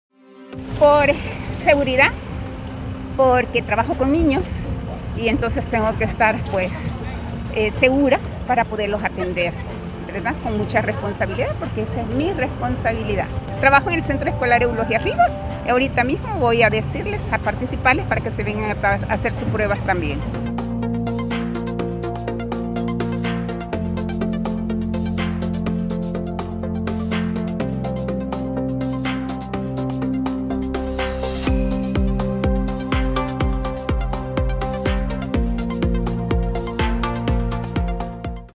Beneficiada con prueba PCR destaca la importancia de realizarlas e identificar el virus
TamizajeComunitario | Por su seguridad, la de su familia y sus alumnos, Marta Argueta, se realizó la prueba COVID19, en Cojutepeque, Cuscatlán.